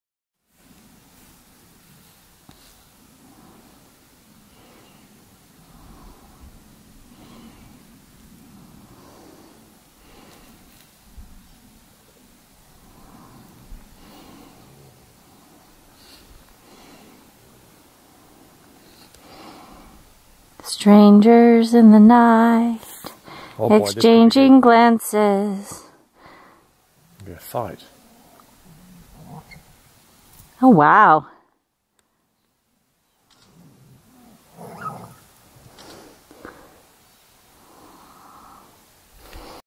Marmot Mating Attempt (part 1)
One marmot wanted to mate and the other wasn't interested, it seems. This was in northern Washington State, on the Pacific Crest Trail.